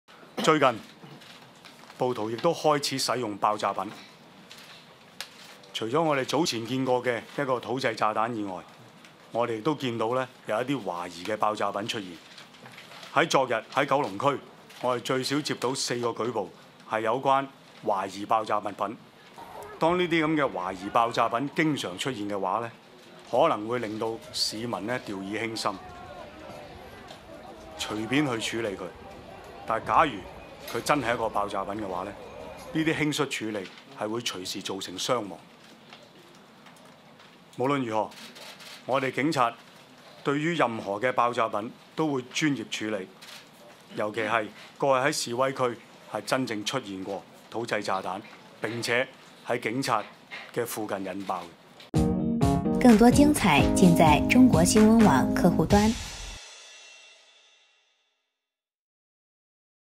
香港警方：10月20日接获至少4宗有关怀疑爆炸品举报
10月21日，香港警方会见传媒时表示，黑衣蒙面人近月四处纵火破坏，最近更有土制炸弹及怀疑爆炸品出现，昨日警方接获至少4宗有关怀疑爆炸品的举报。